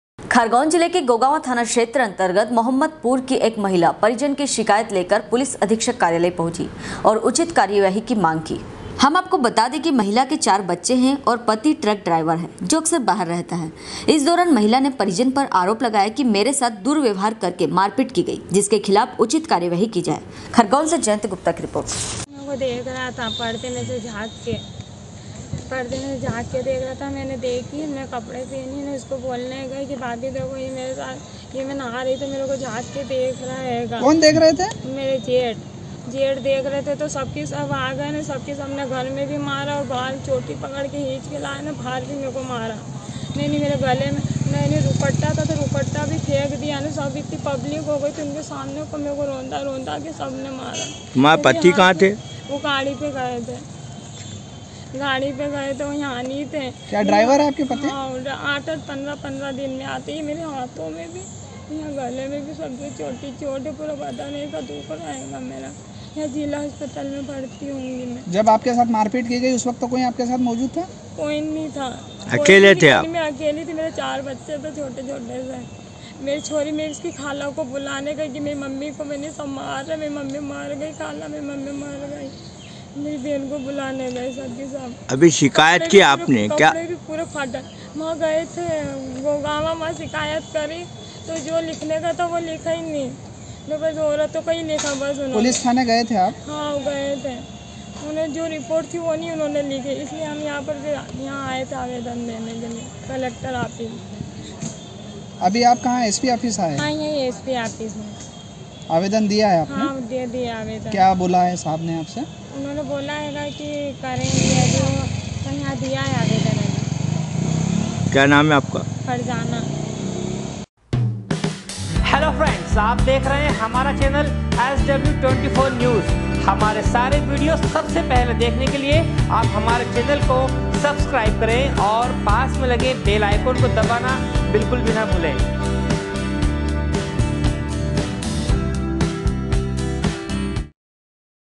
गोगावाँ थाना क्षेत्र अंतर्गत मोहम्मदपुर की एक महिला पुलिस अधीक्षक कार्यालय पहुची SW 24News 09.07.21
sw sw24news sw24newsmadhyapradesh livenews news24खरगोन जिले के गोगावाँ थाना क्षेत्र अंतर्गत मोहम्मदपुर की एक महिला परिजन की शिकायत लेकर पुलिस अधीक्षक कार्यालय पहुची और उचित कार्यवाही की मांग की।
खरगोन से जयंत गुप्ता की रिर्पोट